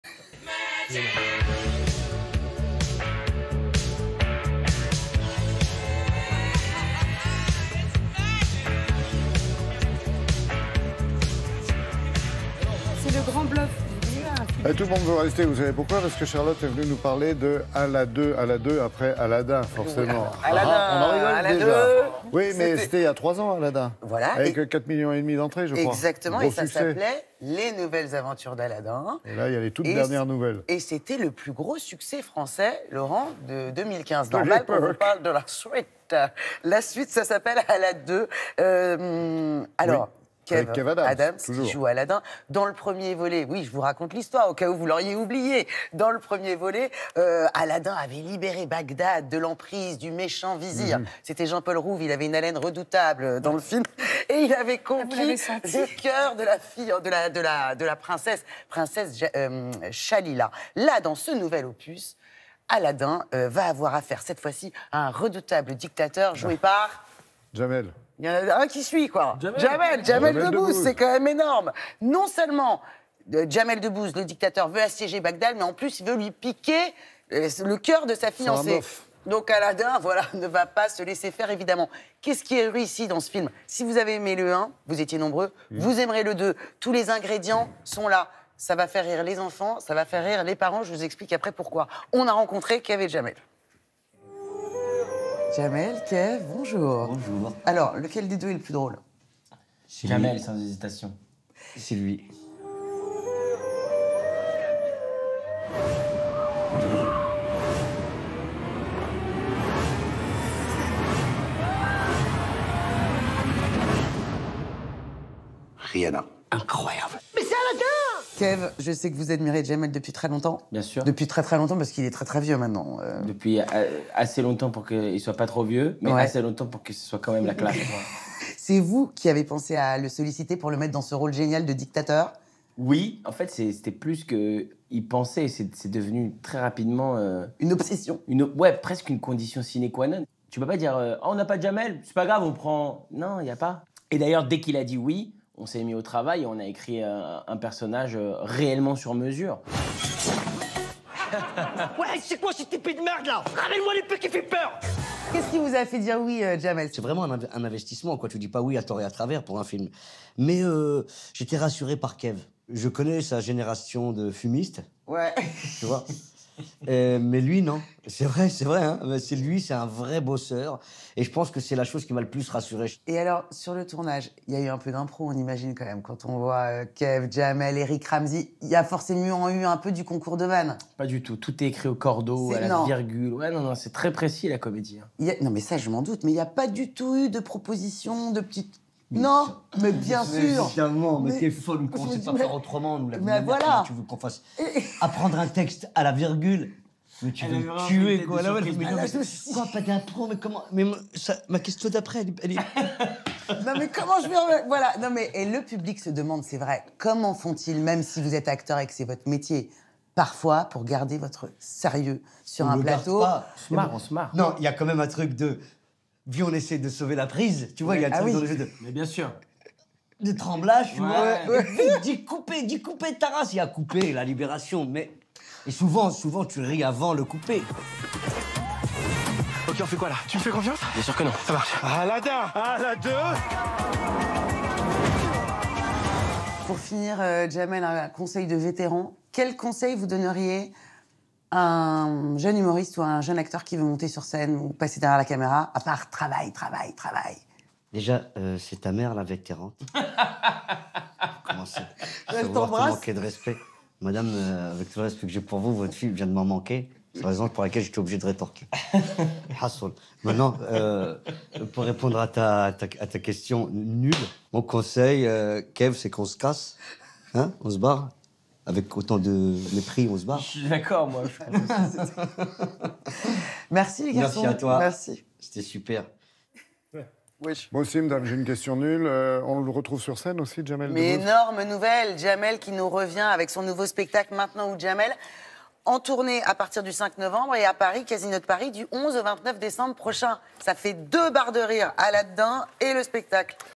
Cinéma – « Alad’2 » de Lionel Steketee
Interview de Kev Adams et Jamel Debbouze